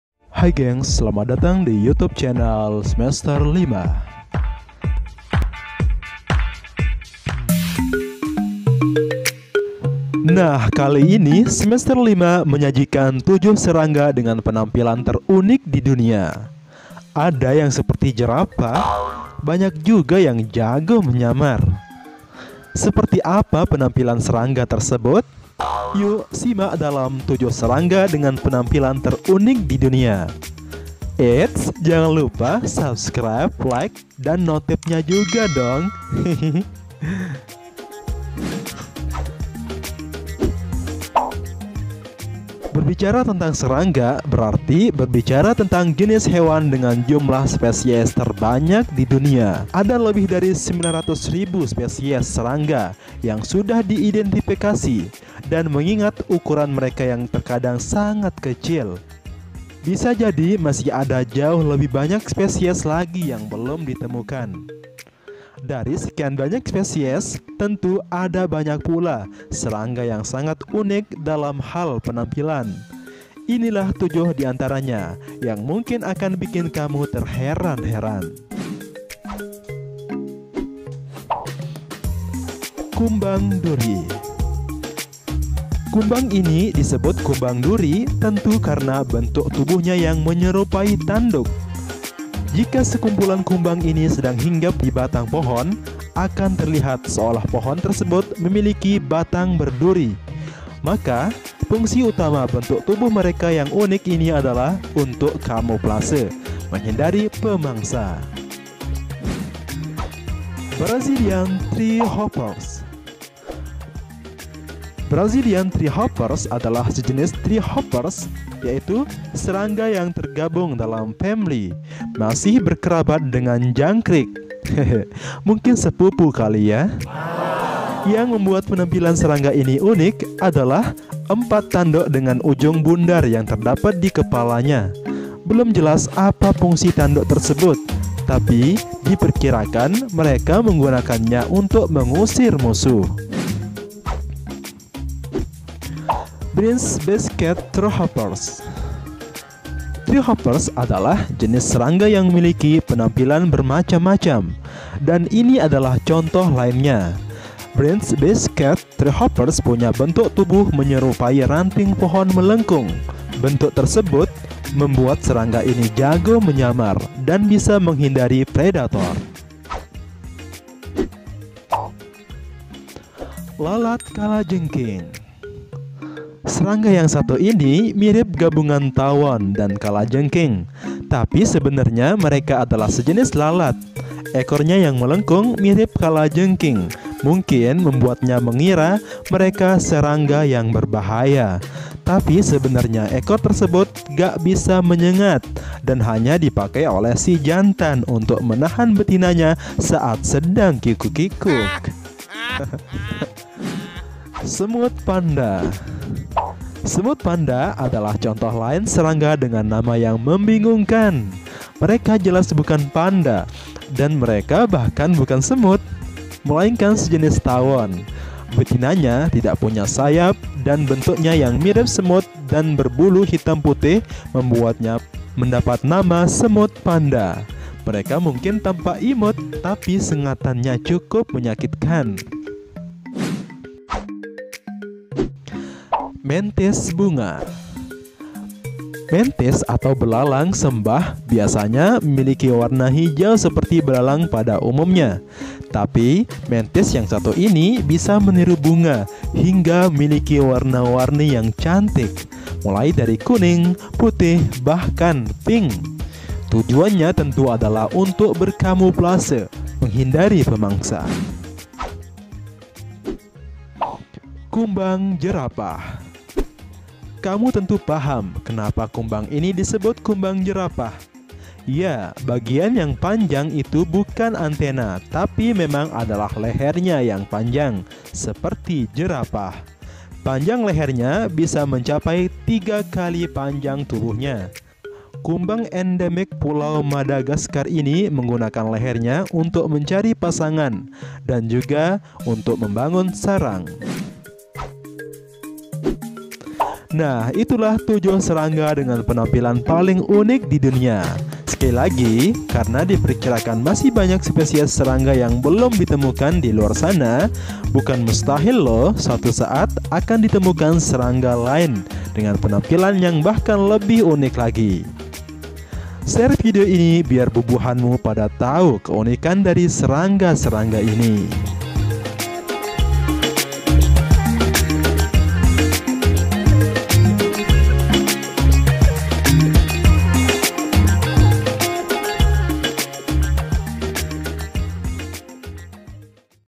7 Serangga terunik di dunia
Nah kali ini semester 5 menyajikan
7 Serangga dengan Penampilan Terunik,di dunia
Ada yang Seperti Jerapah
Banyak juga yang jago menyamar

Berbicara tentang serangga berarti berbicara tentang jenis hewan dengan jumlah spesies terbanyak di dunia. Ada lebih dari 900 ribu spesies serangga yang sudah diidentifikasi. Dan mengingat ukuran mereka yang terkadang sangat kecil, bisa jadi masih ada jauh lebih banyak spesies lagi yang belum ditemukan.

Dari sekian banyak spesies, tentu ada banyak pula serangga yang sangat unik dalam hal penampilan. Inilah 7 di antaranya, yang mungkin akan bikin kamu terheran-heran. Check them out
Sumber info :IDN Times logo